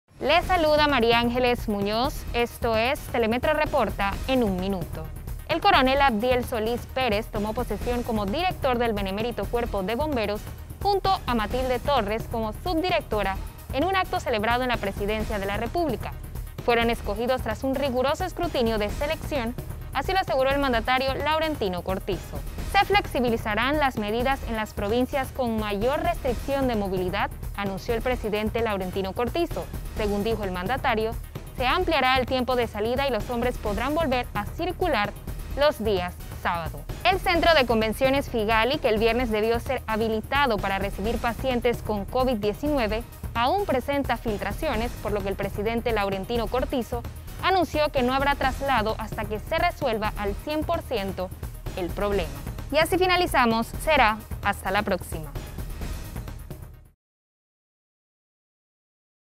TR en un Minuto: 18 de agosto de 2020
Conozca la razón por la cual no se permitirán traslados al Centro de Convenciones Figali en este momento, en el TRenUnMinuto con Maríangeles Muñoz.